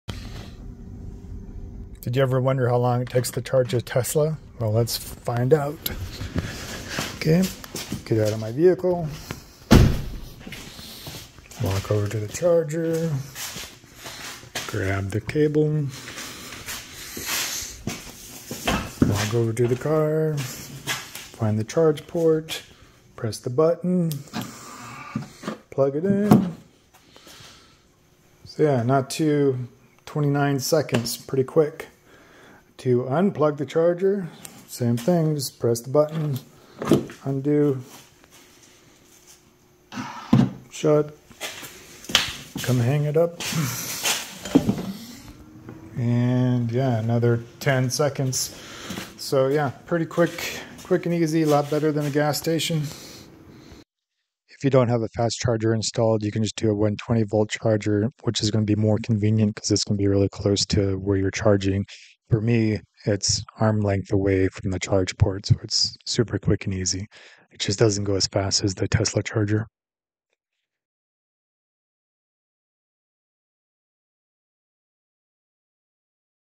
How difficult is it to charge a Tesla?
How to charge a Tesla or any EV at home. How quick or convenient is it to charge at home?  Really quick 20-40 seconds. A gas vehicle would be 5 minutes maybe, plus the time to drive to and from the gas station. In this video I use a Tesla Home Mobile charger and  a CCS to NACS adapter  with a old Voltec Charger from a Chevy Volt. SAE J1772 charging adapter.